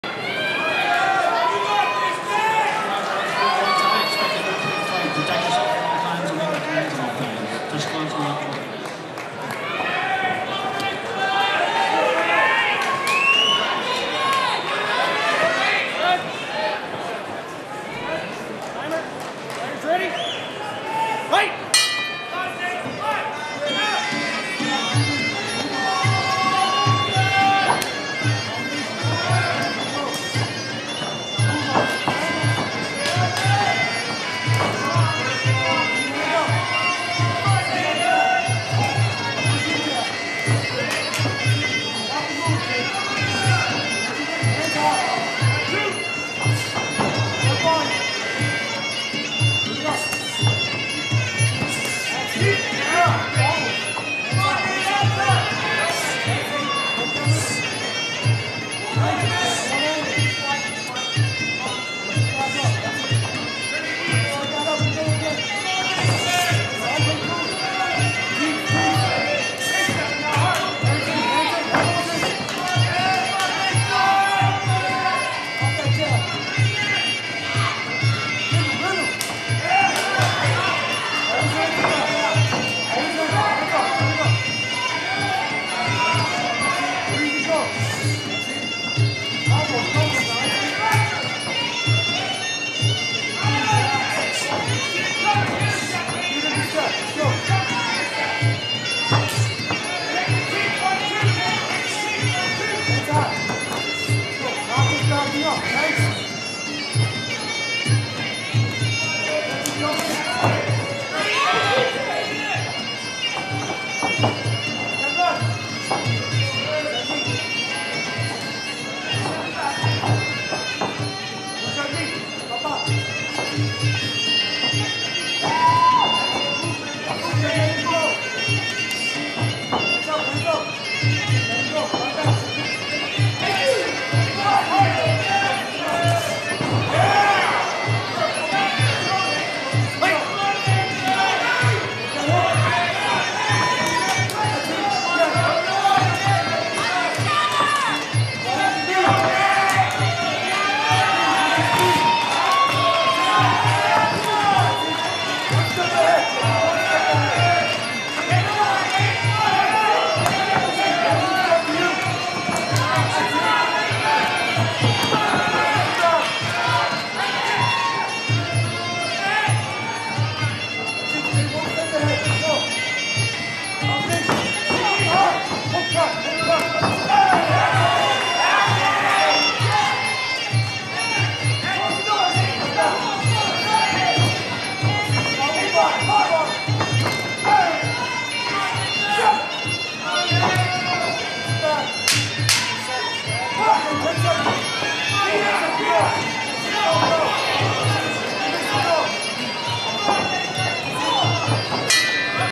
Smandych vs Greskiewicz Round 1
Journey Fight Series II Saturday March 5, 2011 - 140lbs 8 man Professional Full Muay Thai Rules Tournament - Quarter Finals Match - BLUE Justin Greskiewicz (Cool Hearts Muay Thai, USA) vs. RED Nathan Smandych (Team Smandych, Canada)